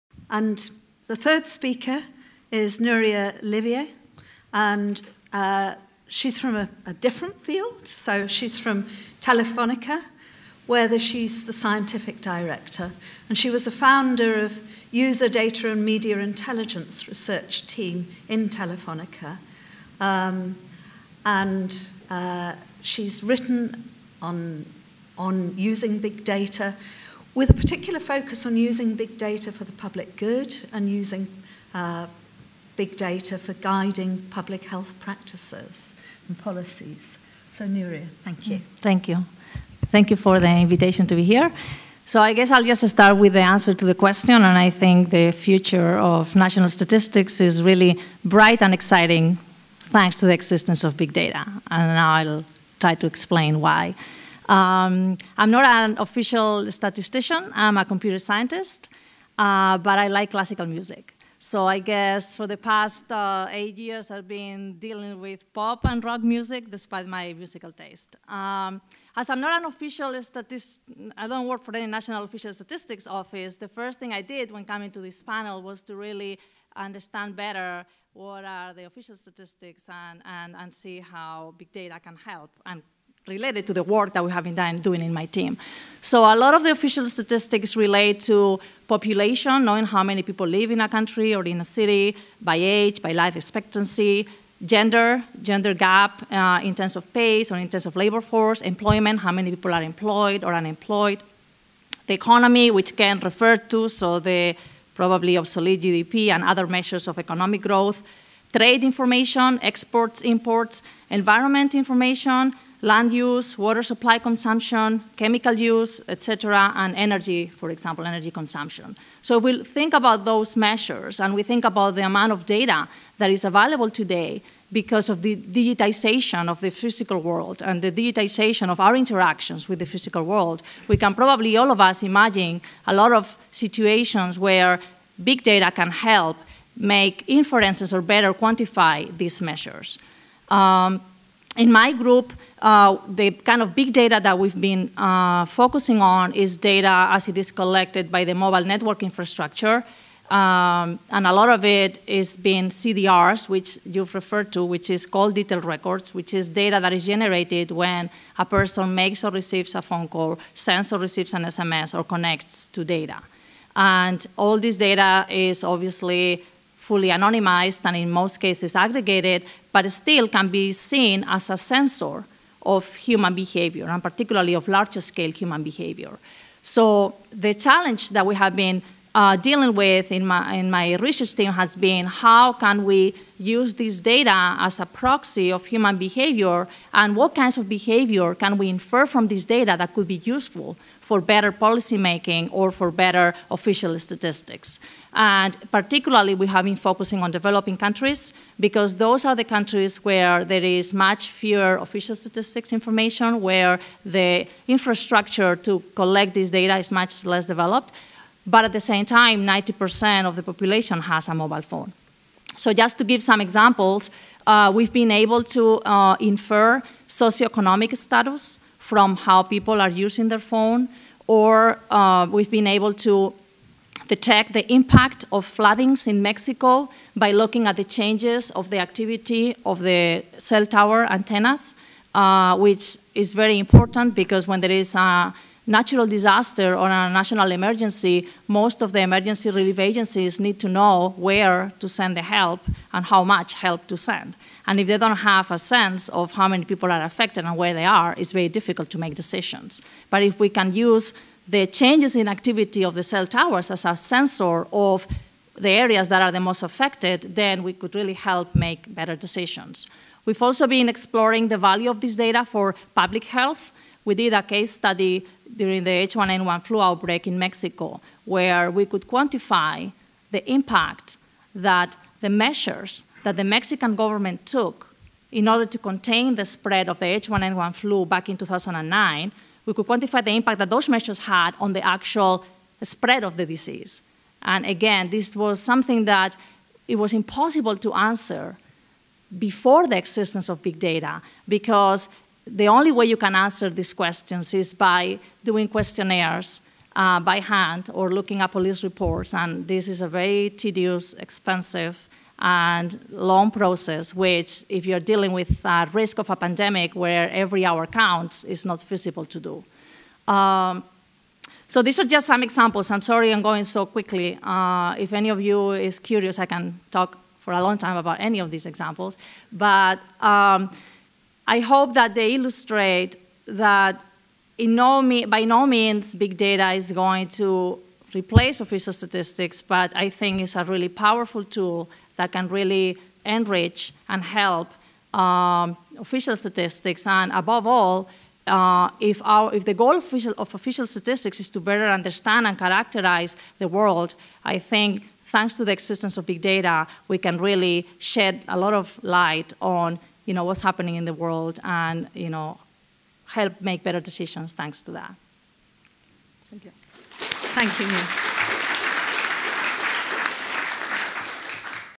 4 Nuria Oliver What is the future of official statistics in the Big Data era?
This public panel discussion was organised by the Overseas Development Institute, Data-Pop Alliance and the Royal Statistical Society. It brought together leading experts who explored the potential of Big Data to transform national statistical systems as well as concerns about the reliability and representativeness of these data, and over ethics, privacy, and the blurring of lines between formal and informal data sources.